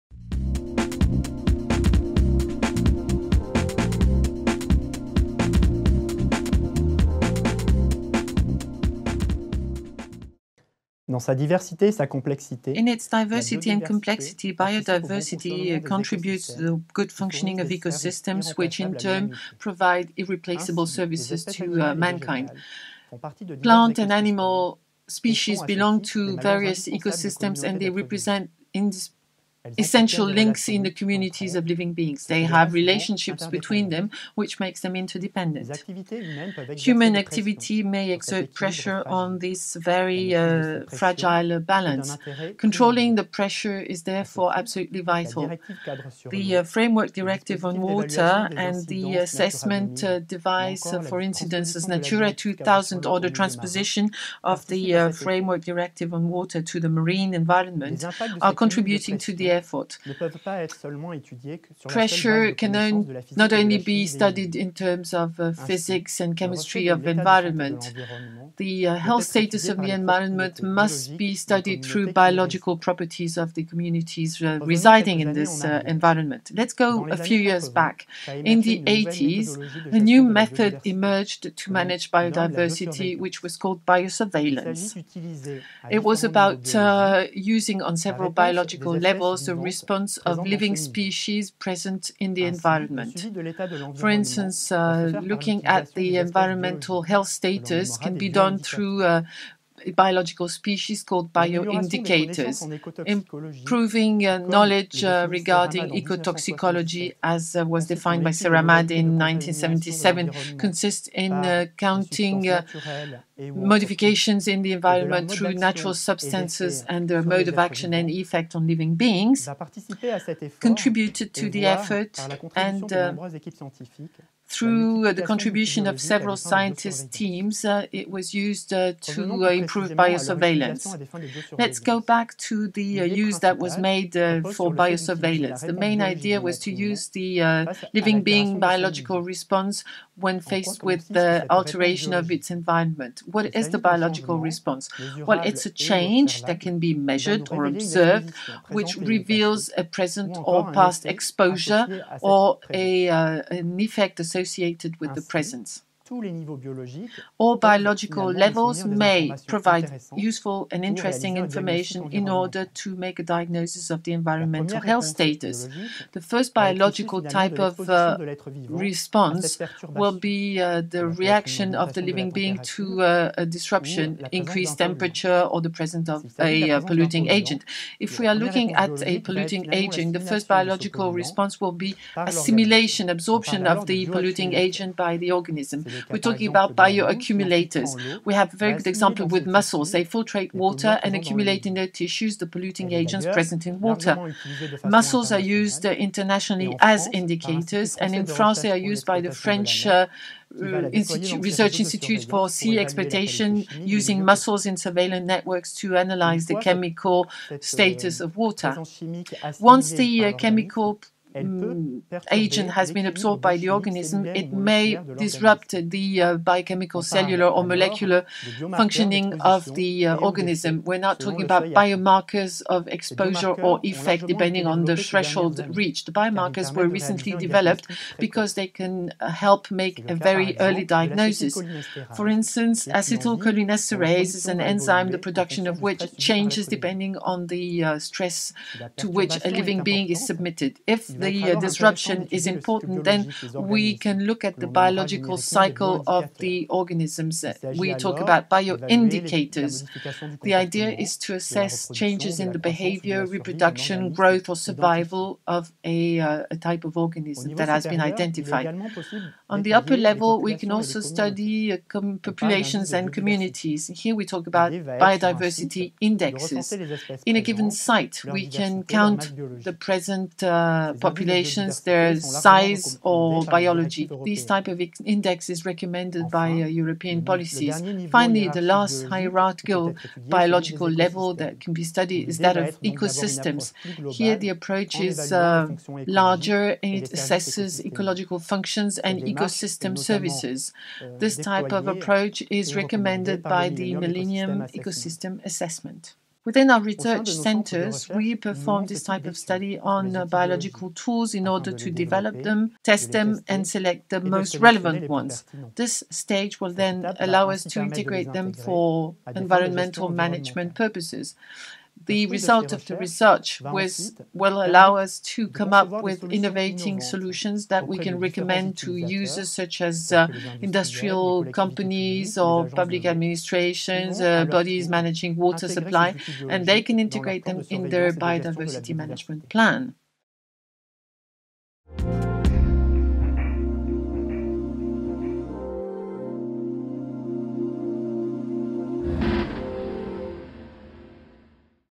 Biomonitoring
Stellio Casas presents the biomonitoring. This tool of biodiversity management can be used for the different biological levels of the life. It leads to the identification of bioaccumulators, biomarkers and biodiversity indications.